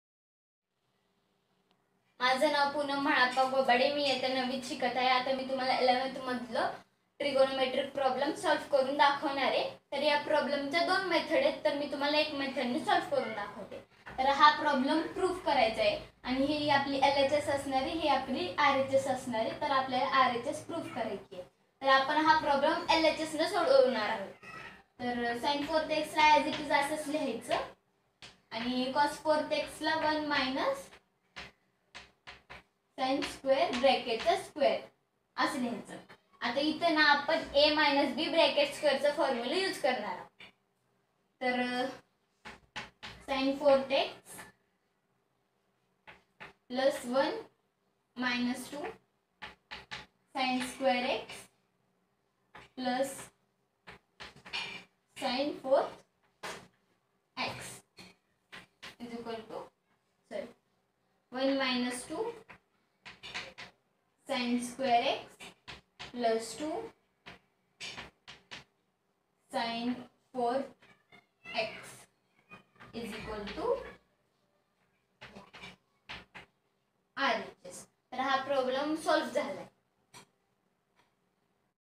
9th class student of IIT/ AIIMS Foundation Online batch solving 12th Trigonometry problem
To join online/ offline foundation batch